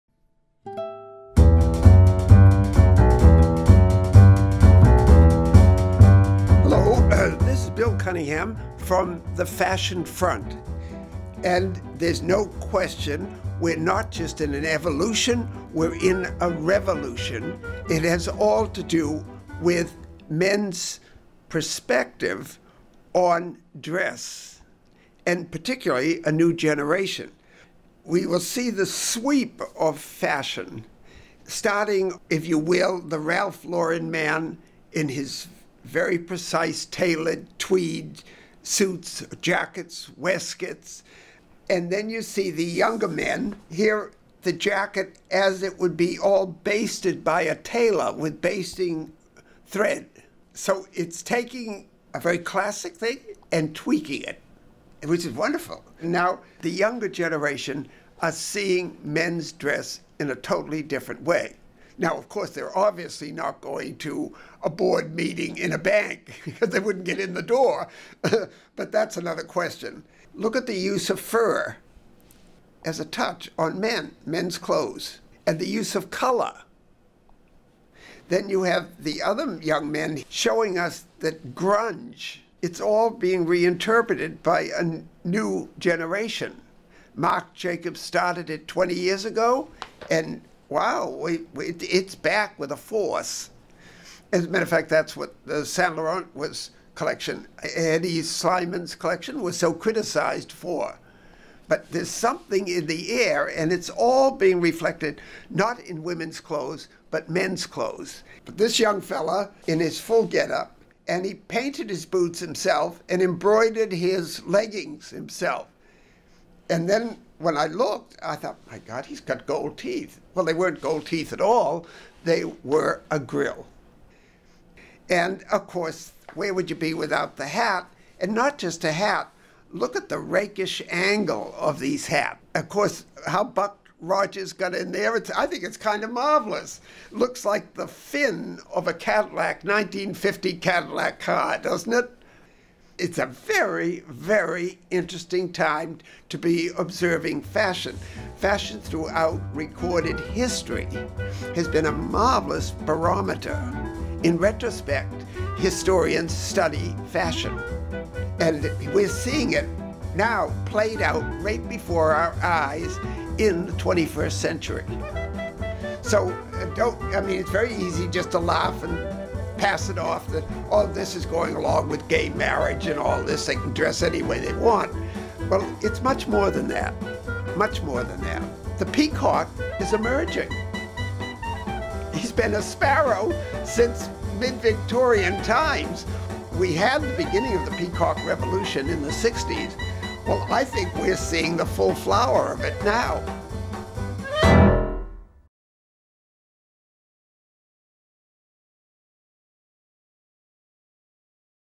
The New Man: On the Street With Bill Cunningham | The New York Times
The men's fashion revolution is in full swing, with looks ranging from '30s styles to a revival of grunge and mouth grilles.